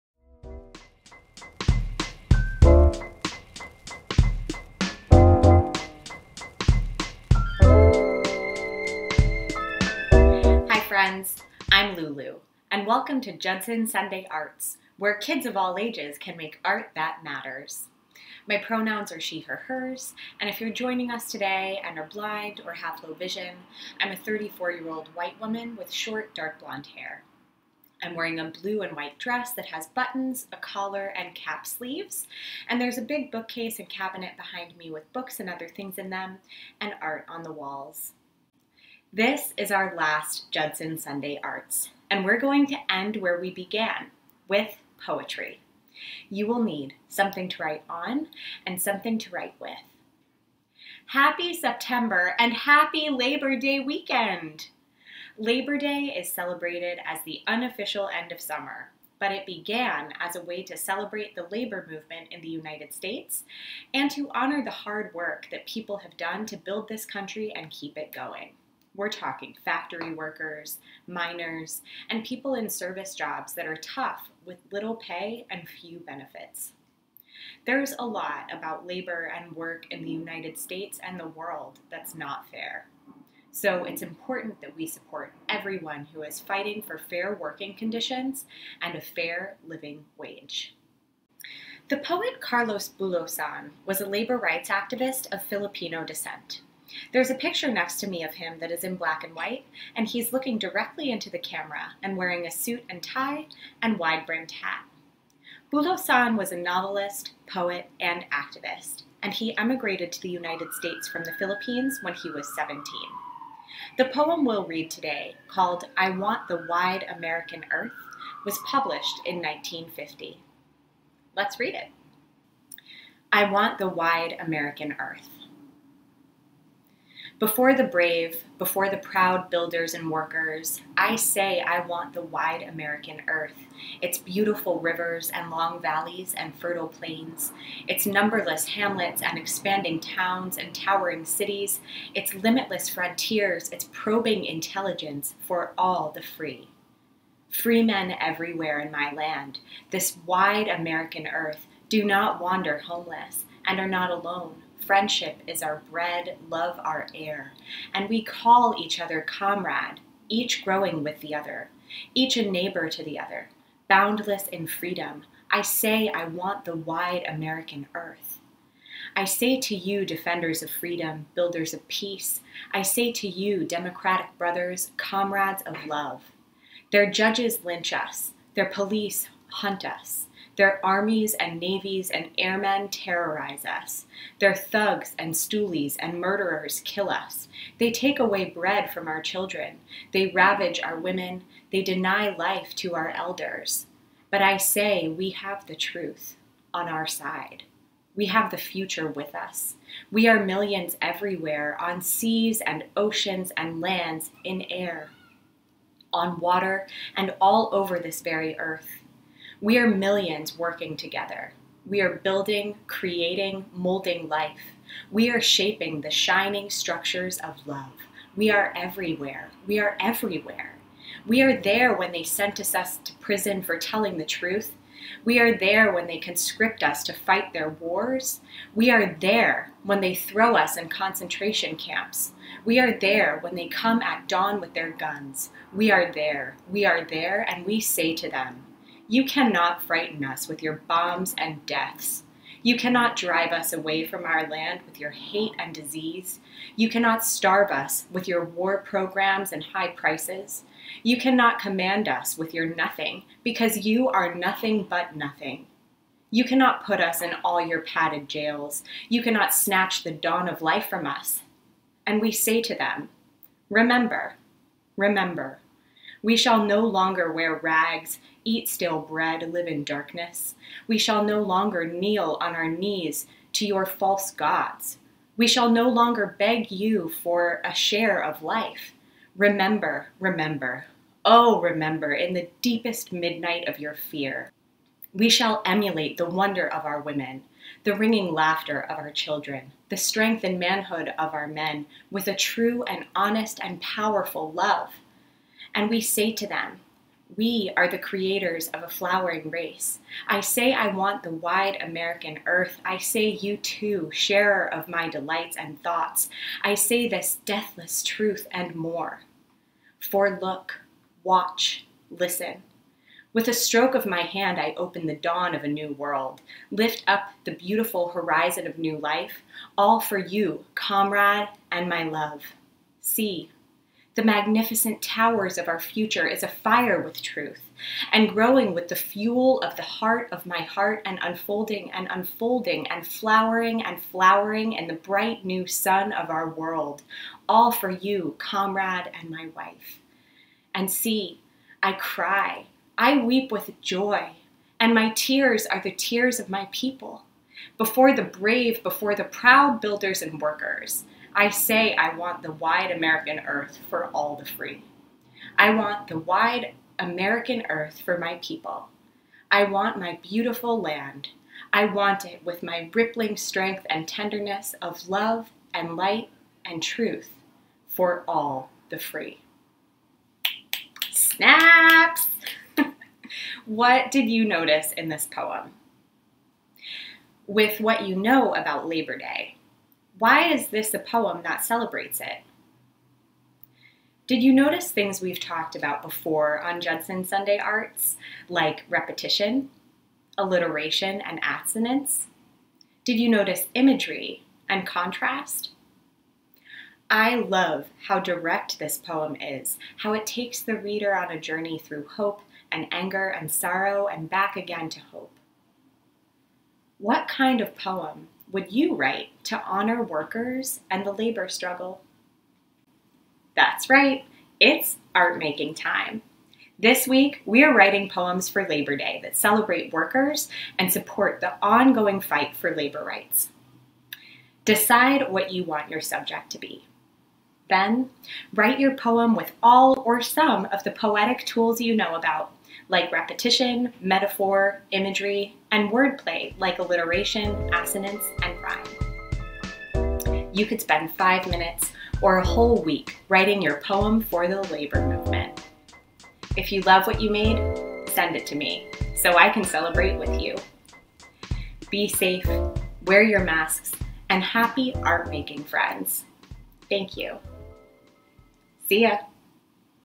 09.05.2021 Judson Sunday Arts: Labor Movement Poetry Writing
Write a poem for Labor Day inspired by Carlos Bulosan!

Let's make art that matters!

This art-making activity is a stand alone addition to the virtual Judson Sunday School collection.